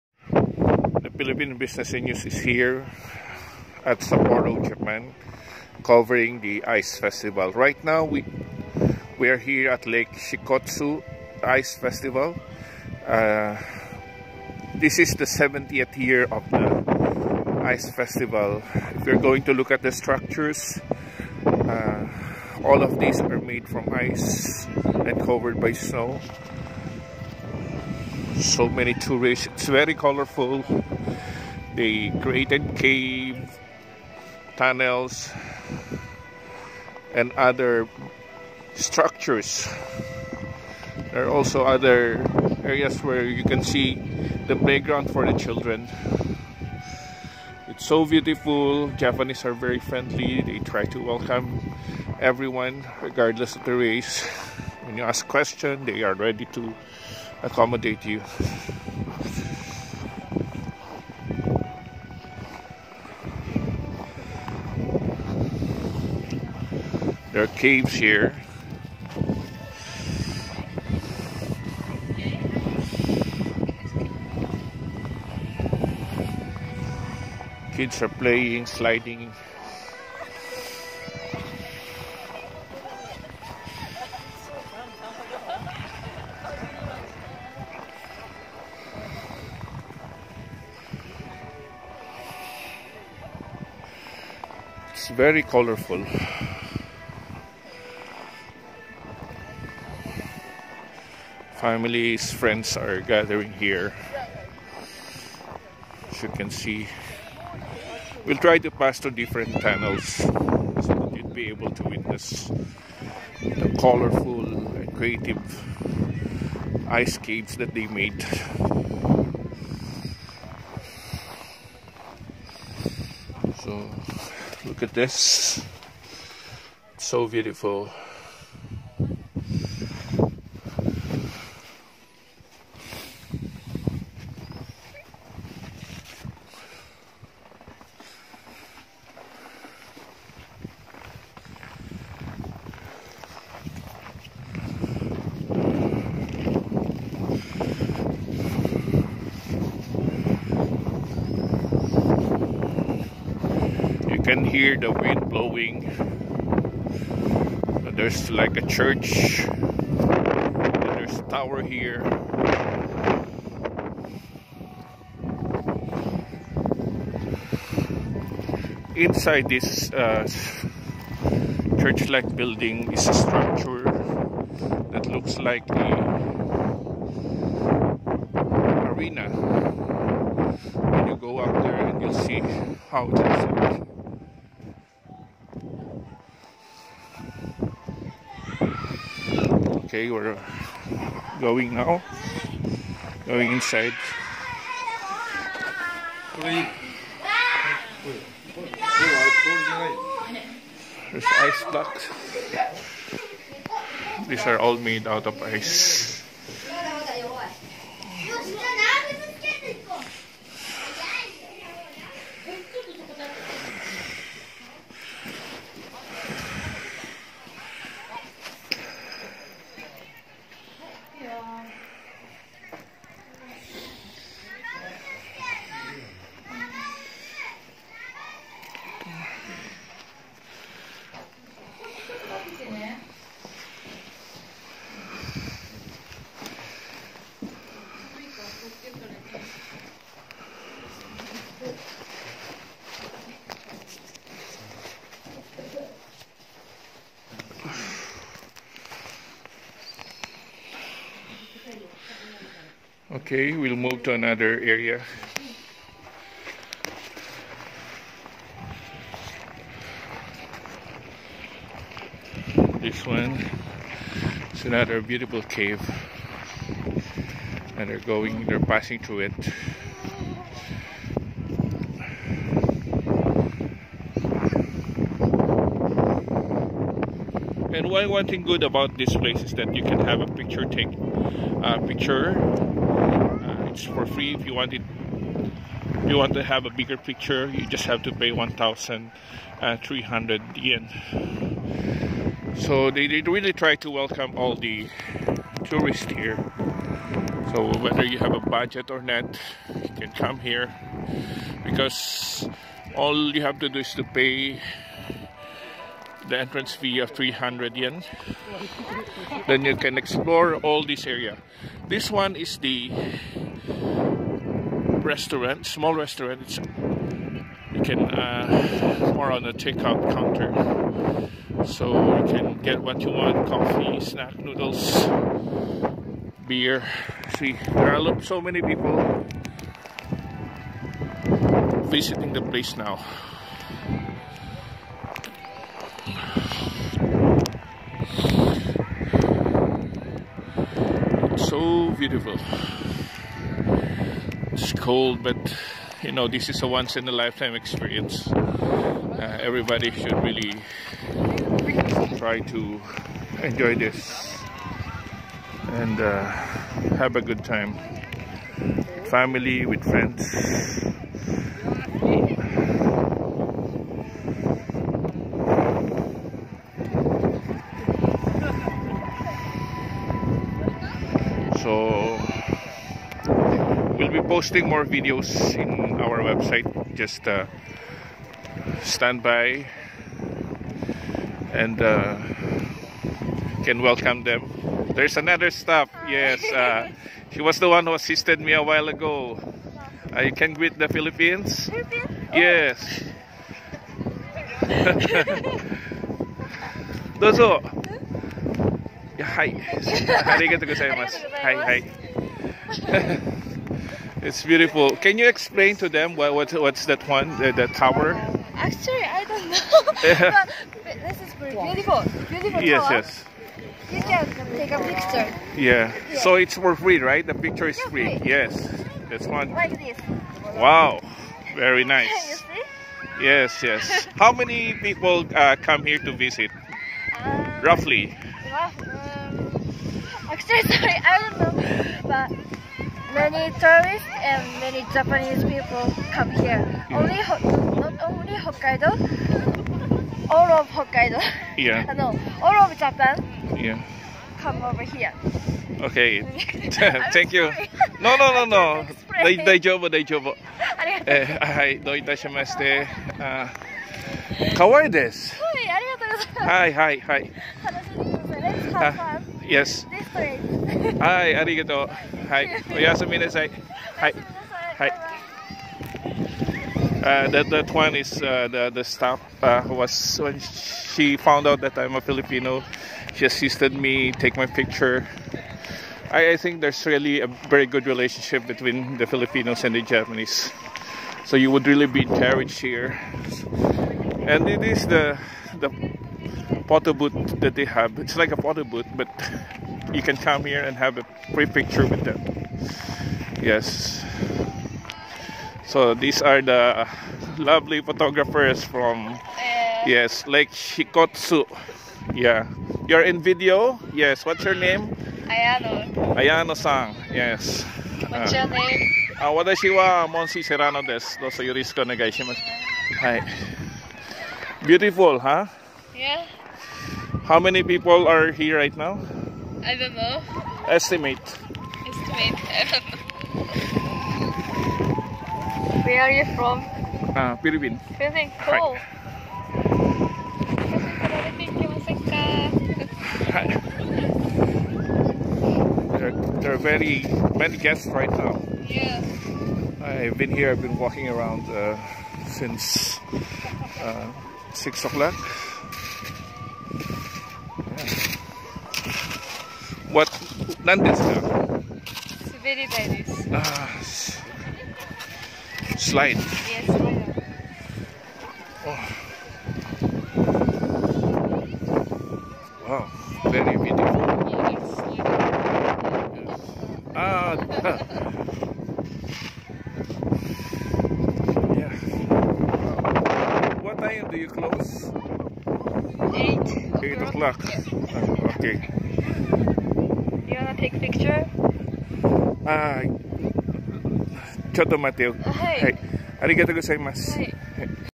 Lake Shikotsu Ice Festival 2019 Covered by THEPHILBIZNEWS
Lovely and colorful Ice Structures created for Lake Shikotsu Ice Festival 2019 being covered by The Philippine Business and News